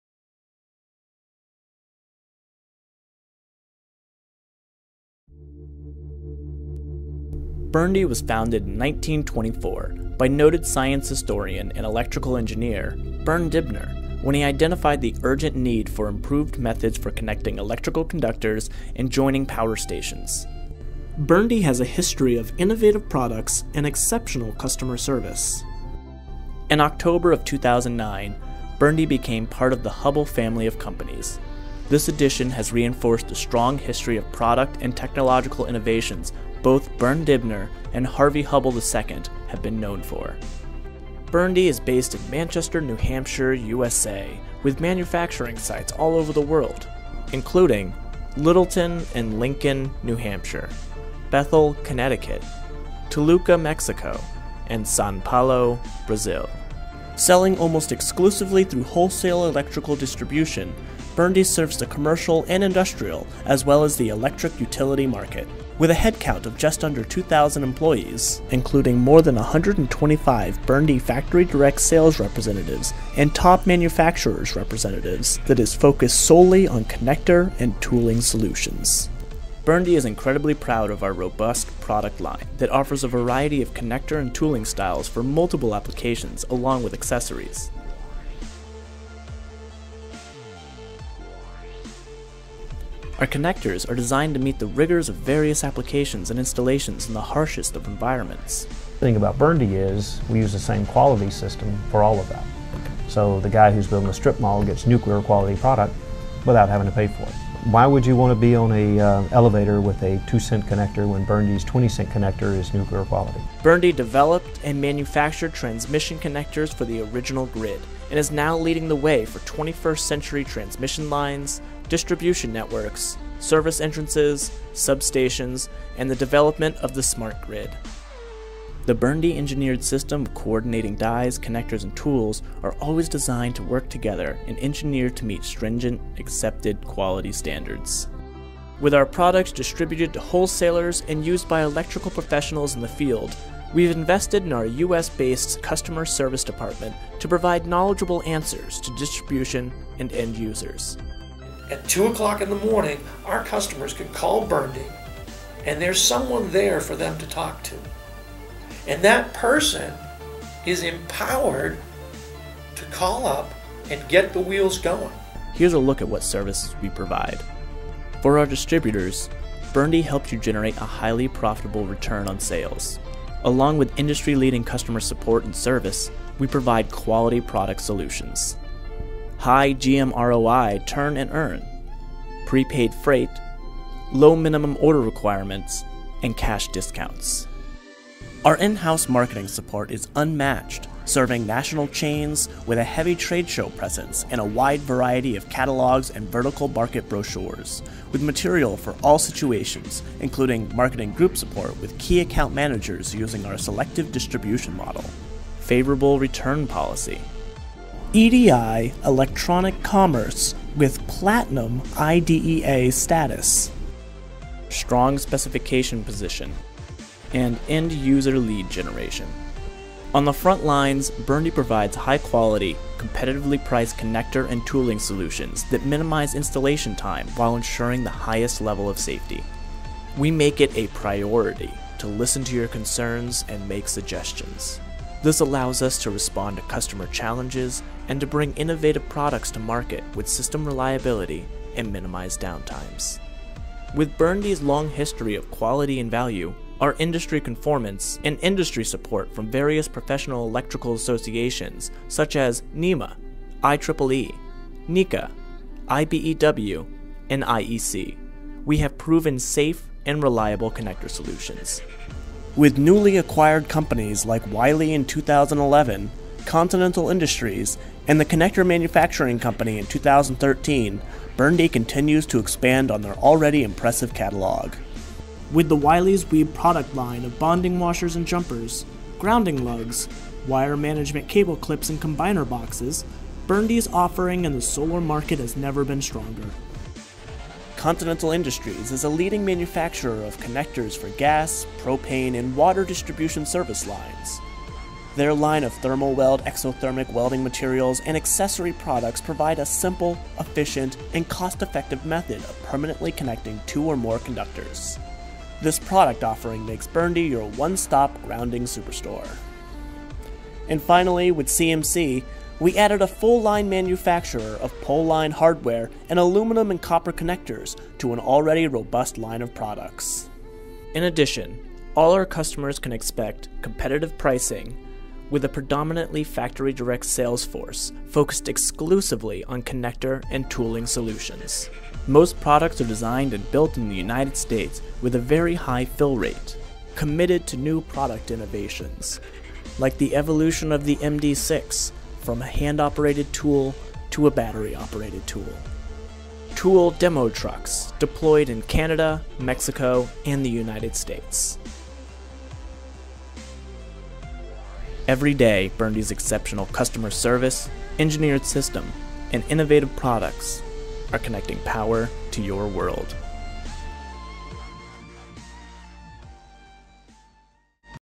BURNDY Capabilities
Since 1924 BURNDY has been know for its high quality, engineering excellence and outstanding customer service. Learn more about BURNDY in this information video.